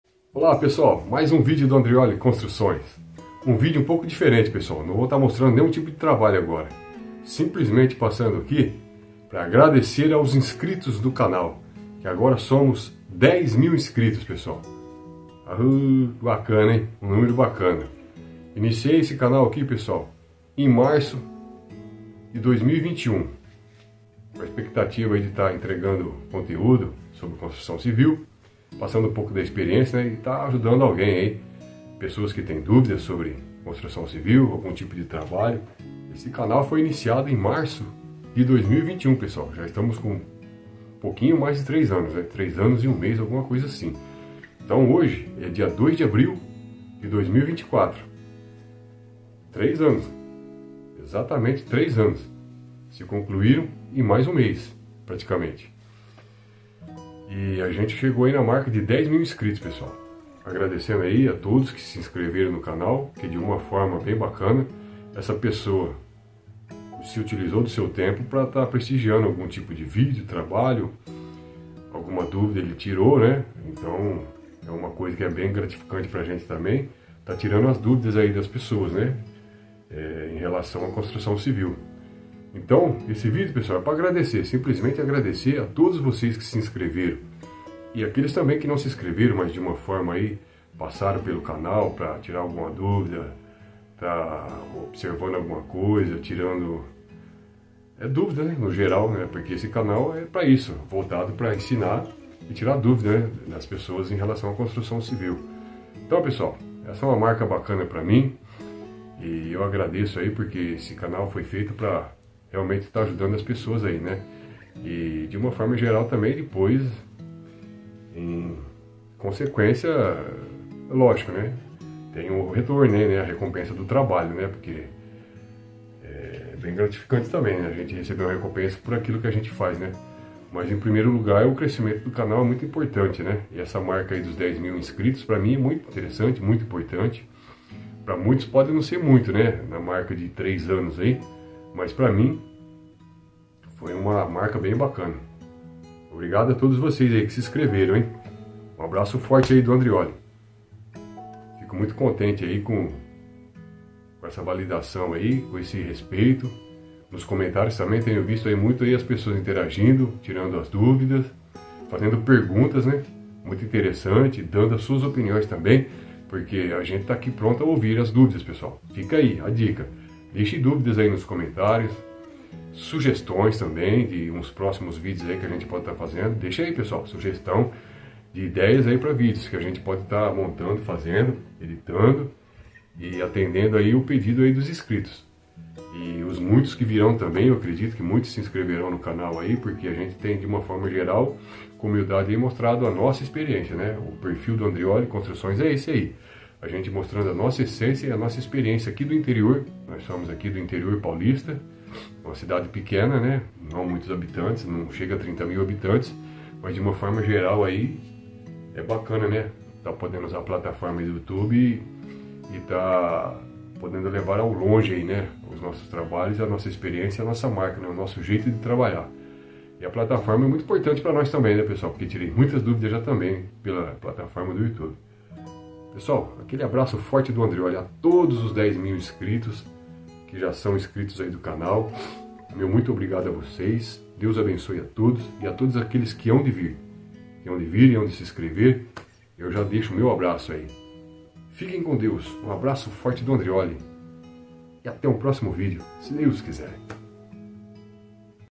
Agradecimento aos inscritos do canal (325)
Este vídeo é um agradecimento aos 10.000 inscritos do canal nesta data